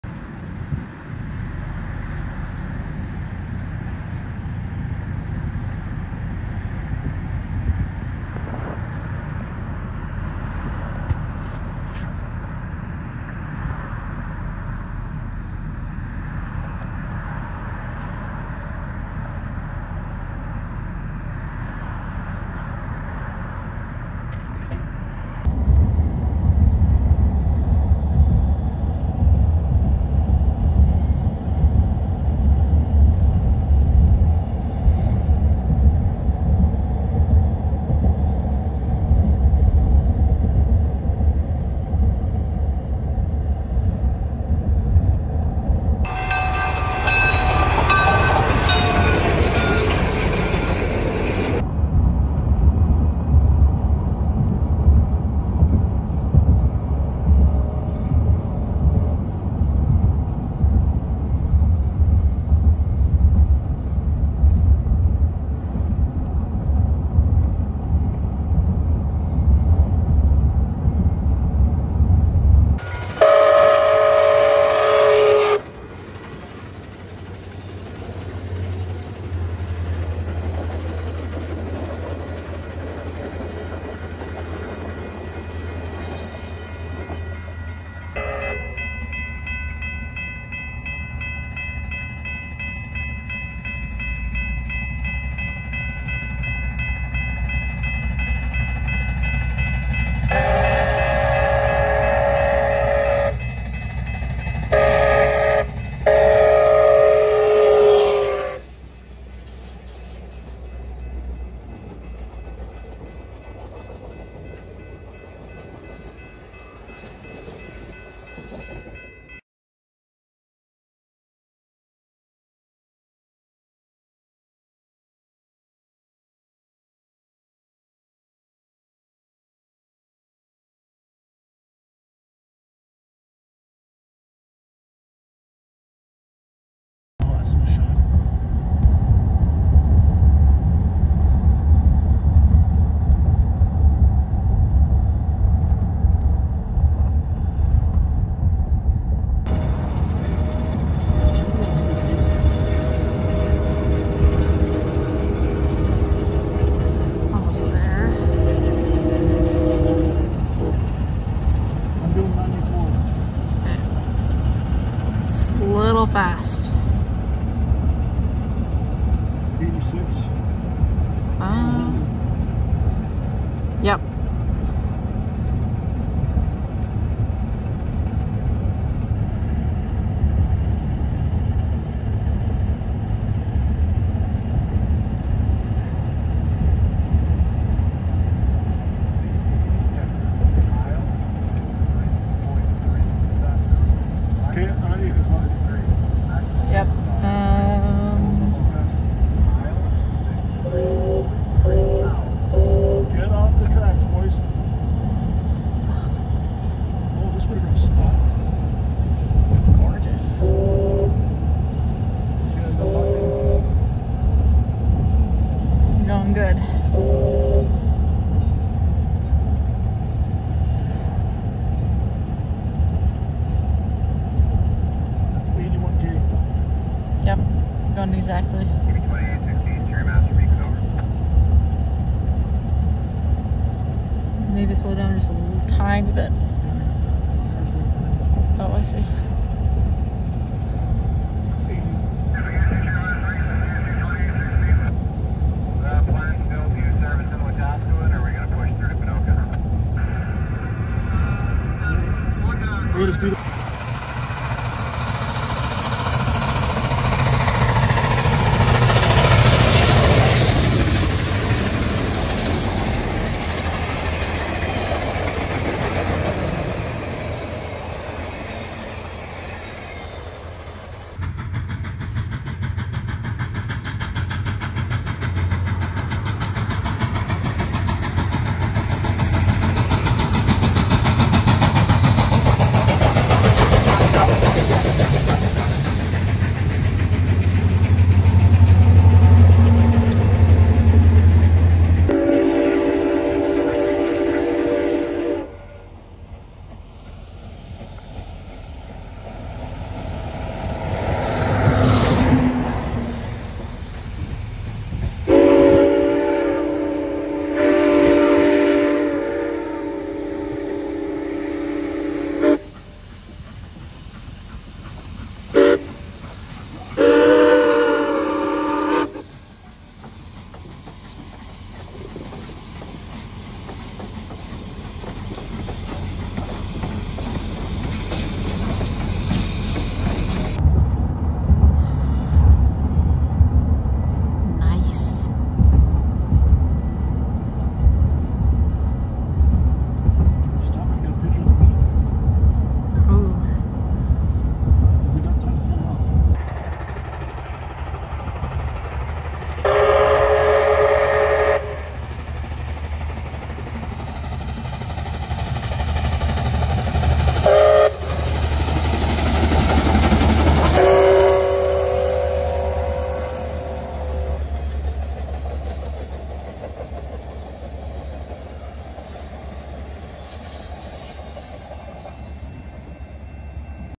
CP 2816 Chase from Edmonton to Red Deer
Had such a great time with a good friend chasing 2816 all the way down to Red Deer. The constant game of cat and mouse, the long brigade of railfan vehicles, her gorgeous whistle, all of it was amazing. Here’s near all of the videos I captured whilst the hunt was on.

CP 2816 Royal Hudson